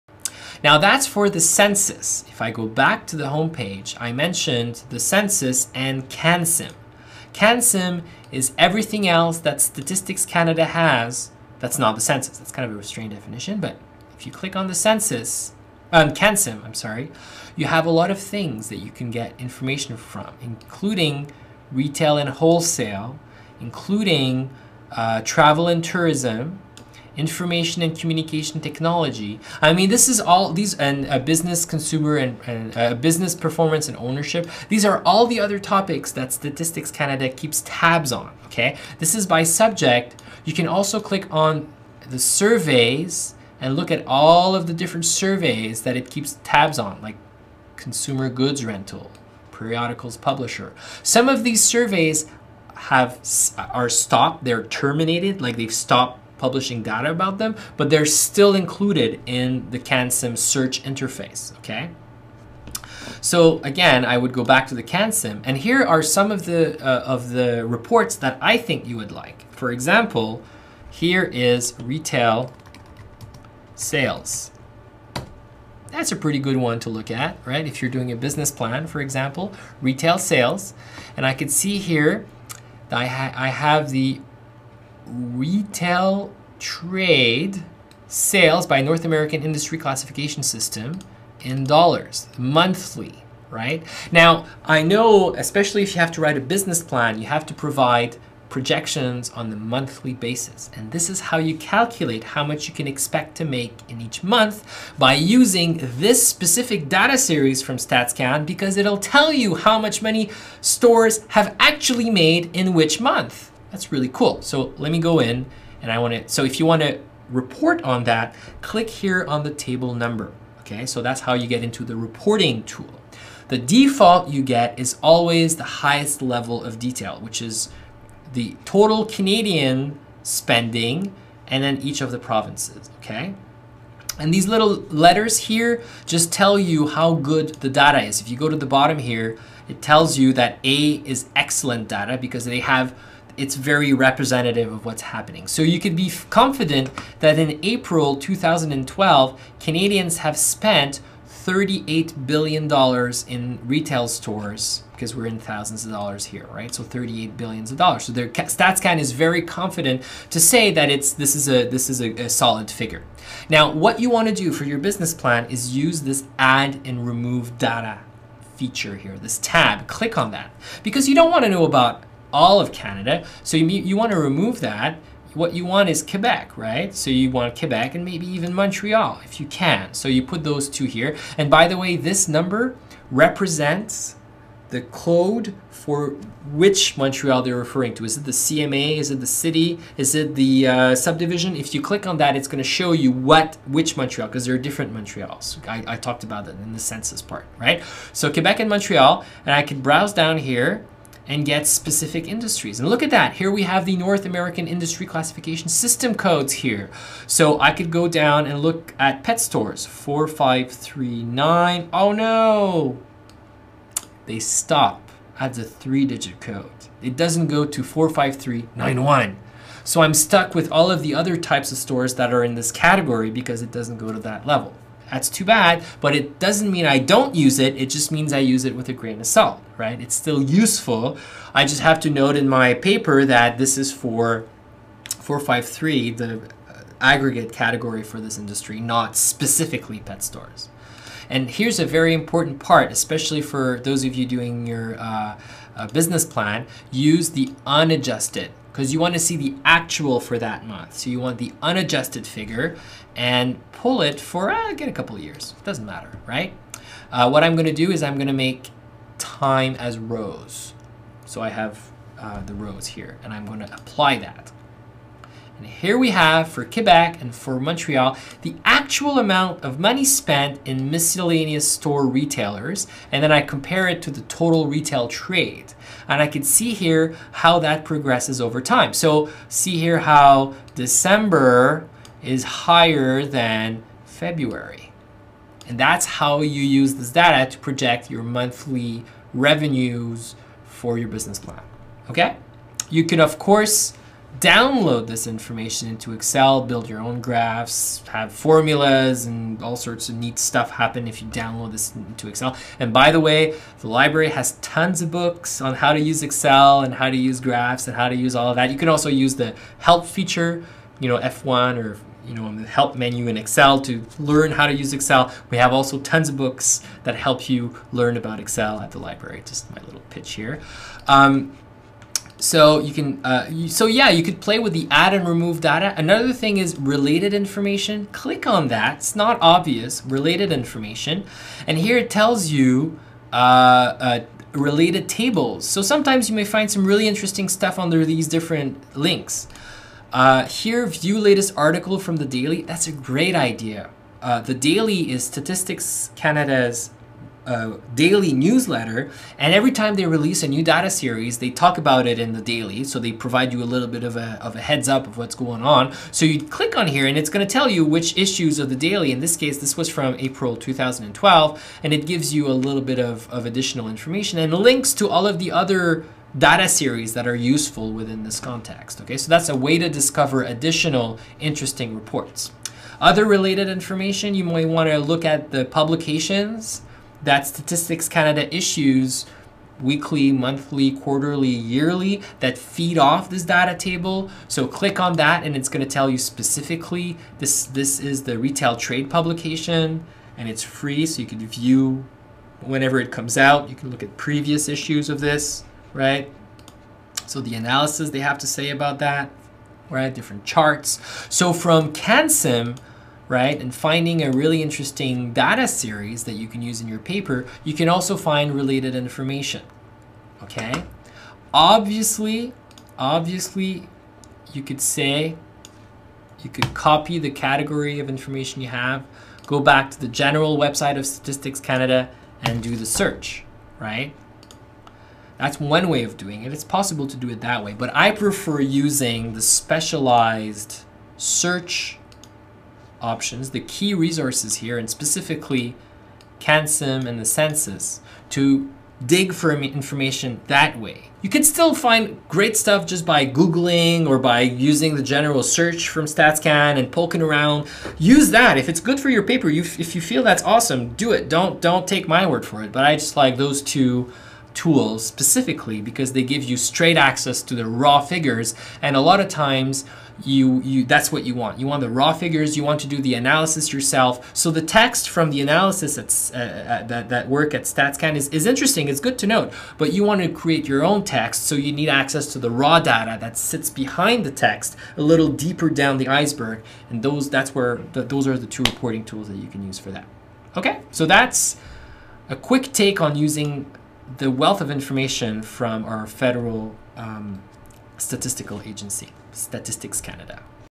CANSIM from Statistics Canada
CANSIM - the CANadian SIMulator - provides access to all other aggregated data produced by Statistics Canada. There are tons of data series, such as on "household spending", "Monthly Retail Sales" and the "Labour Force Survey".

File Code: BR09-2